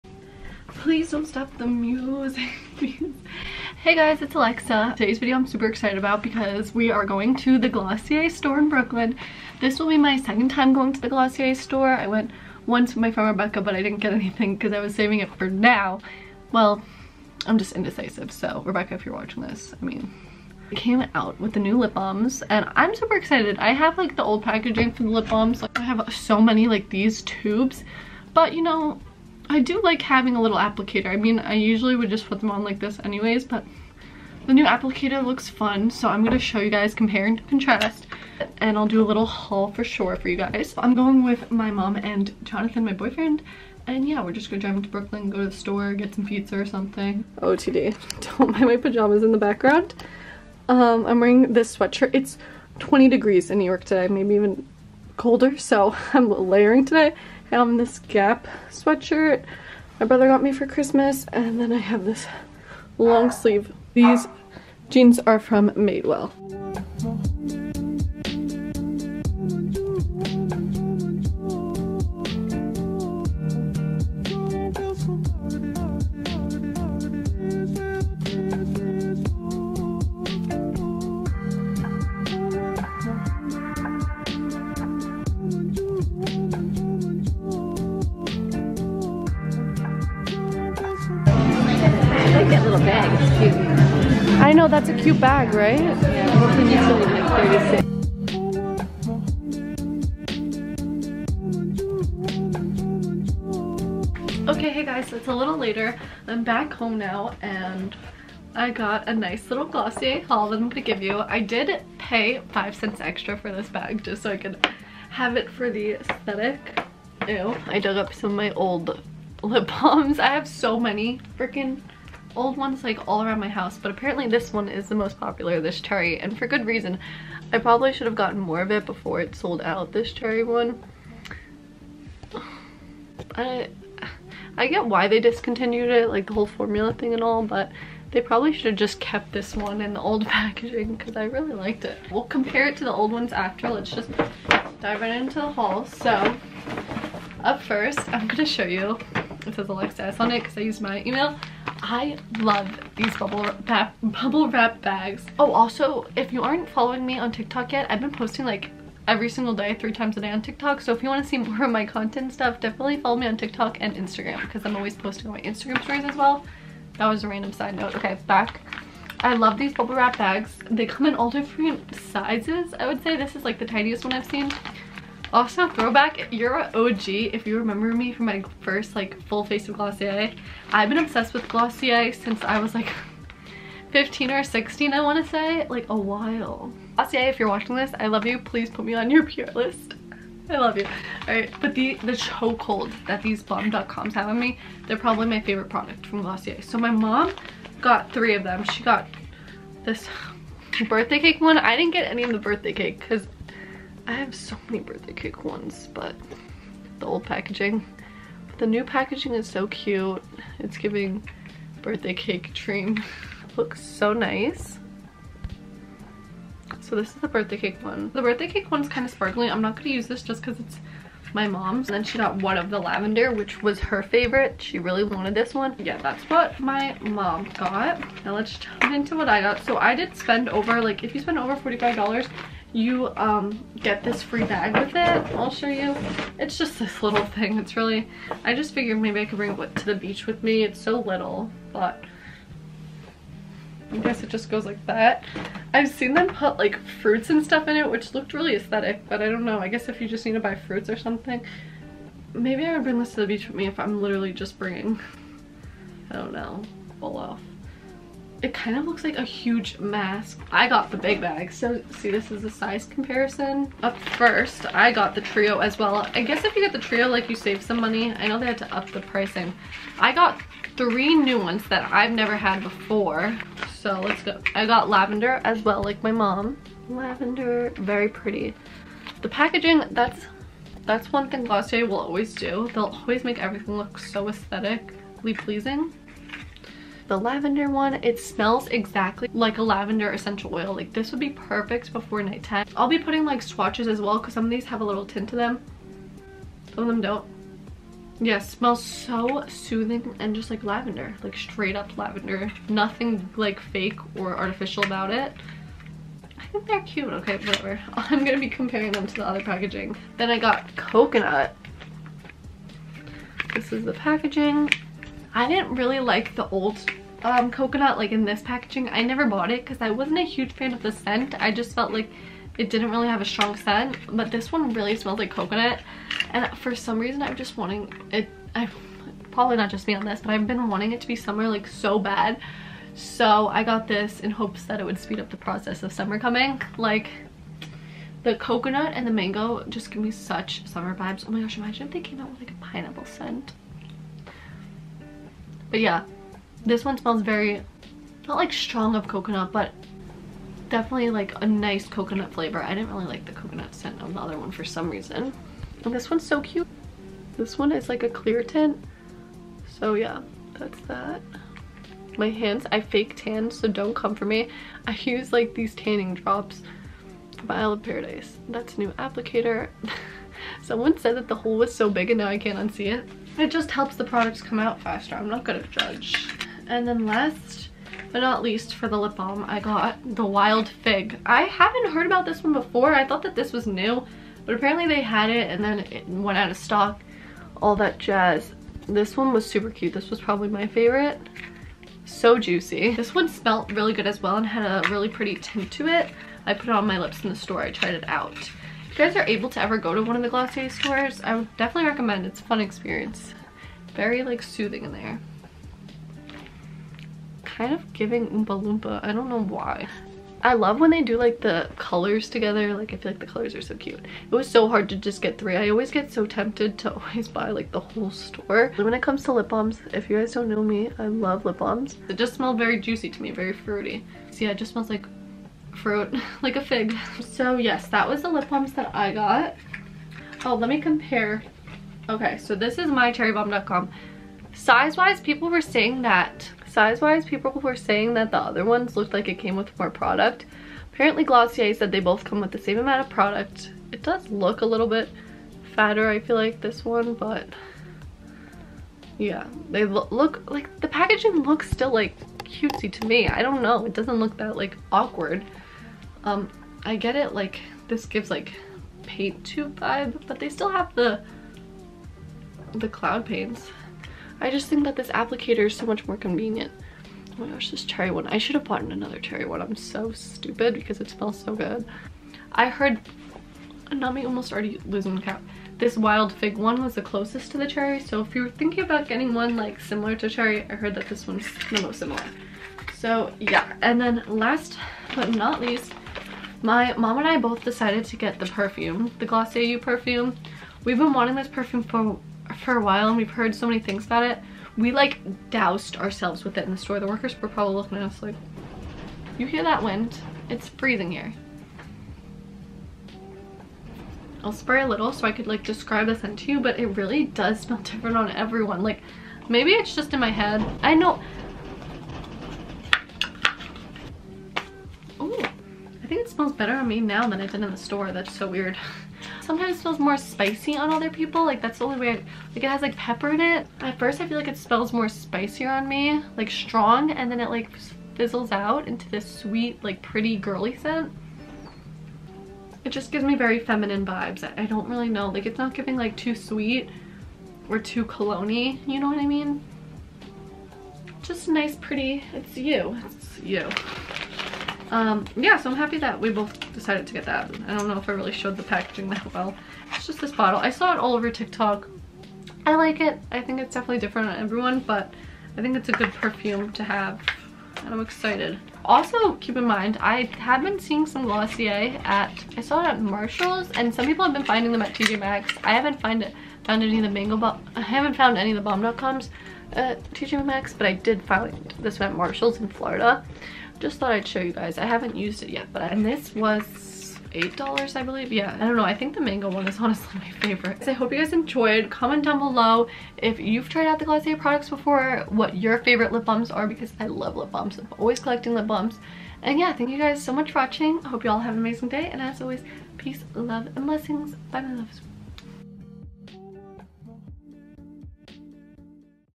TRYING OUT THE NEW GLOSSIER BALM DOTCOMS +nyc brooklyn glossier store and haul!
GLOSSIER VLOG AND HAUL!! Come with me to the glossier store in NYC Brooklyn and see my first impressions to the new glossier balm dotcom lip balms and more:)

I have been posting a lot more on my Tik Tok if you want to check me out on there!! xoxo

Goodreads account:alexaskoros